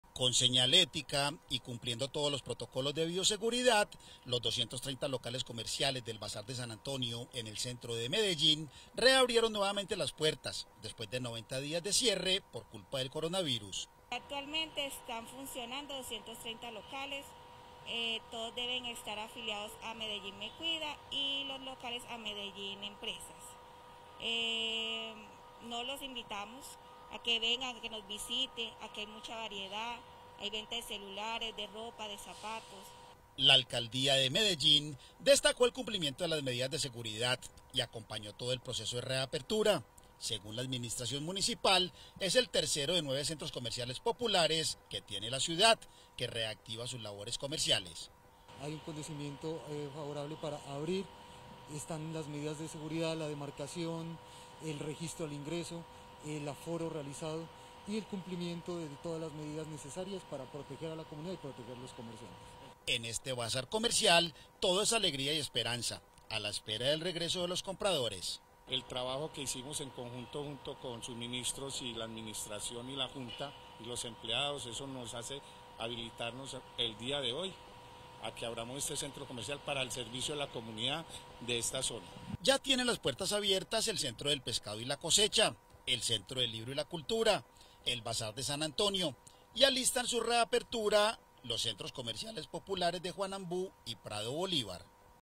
El centro comercial Popular San Antonio reabrió sus puertas - Telemedellín
Cumpliendo con todos los protocolos o medidas de bioseguridad, reabrió las puertas desde hoy, el centro comercial Popular San Antonio que genera cerca de 800 empleos en 230 locales comerciales.